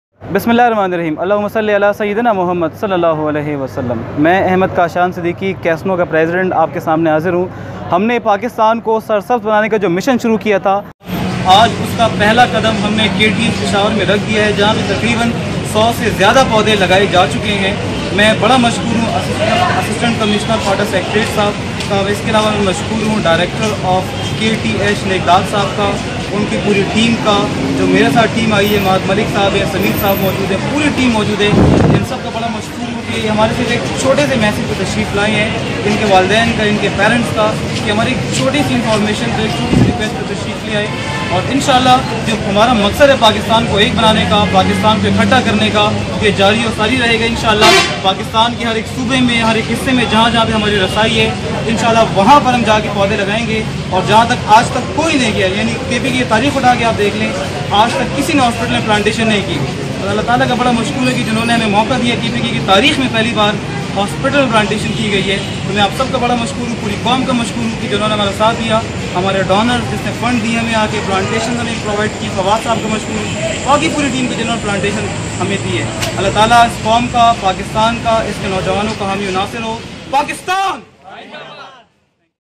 Thank u all
Thank u so much All the Govt officials and other honorable guests for joining me at KTH for Planation

Upcoming plantation drive shall be commence at LRH in coming week..

Son of soil